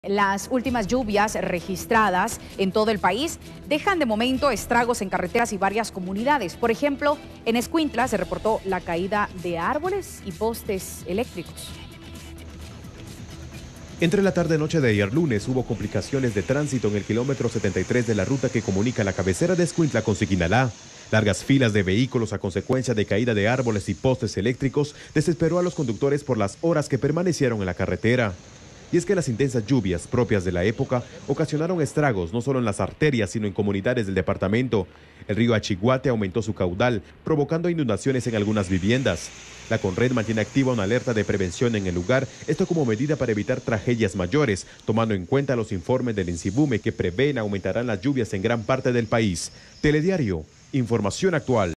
Lluvias fuertes han causado estragos en el país
En Escuintla se han registrado daños en las vías públicas por caída de árboles y postes eléctricos debido a las fuertes lluvias en el área. Ríos han crecido y la CONRED se mantiene en alerta